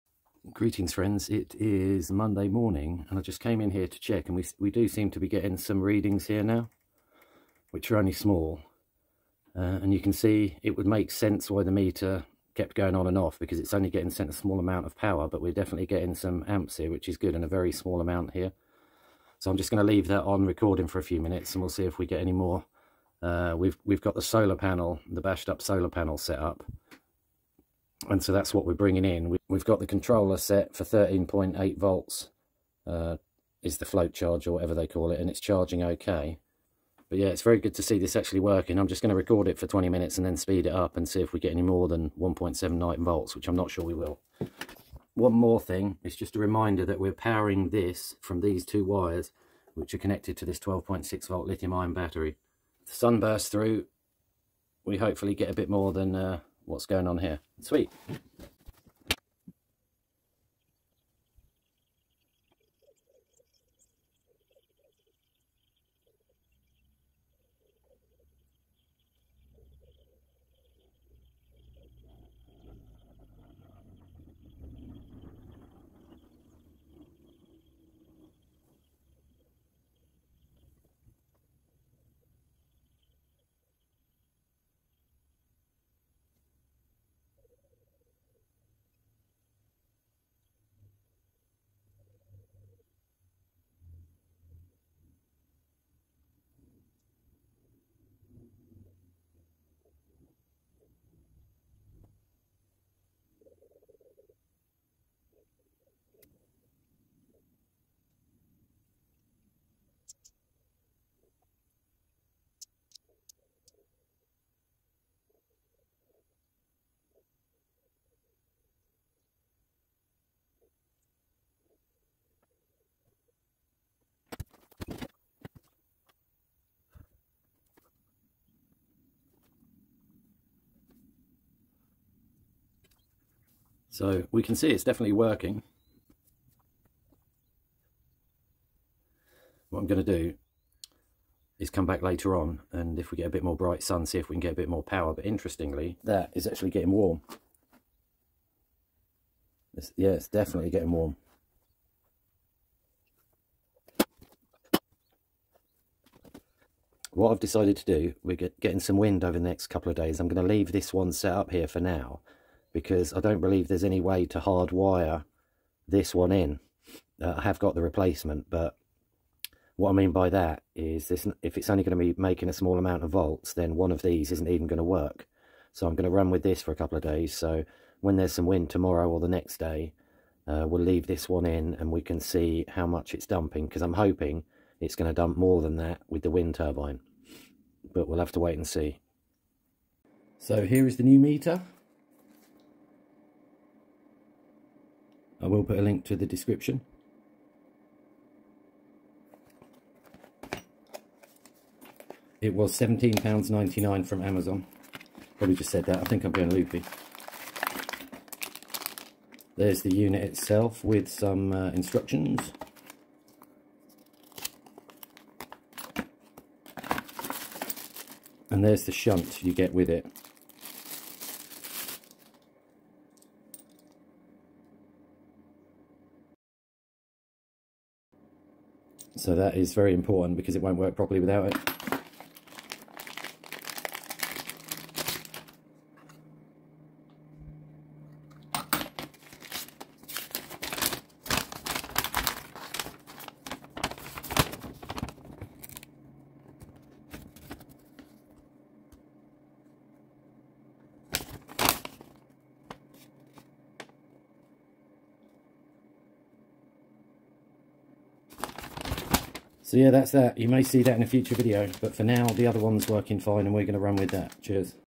Second Meter ( for dump load ) is now working perfectly with Solar Panel - New Meter arrived
Hello, I am very happy this appears to be working perfectly now that it's hard wired to a 12v supply that the meter stays on all the time, due some wind in the next few days looking forward to testing it then. Please note the dump load is getting warm not sure why will such a small amount of power going through it.

I am planning on leaving this meter connected for a few days see how it functions with the wind.

link Below for the new watt meter that I purchased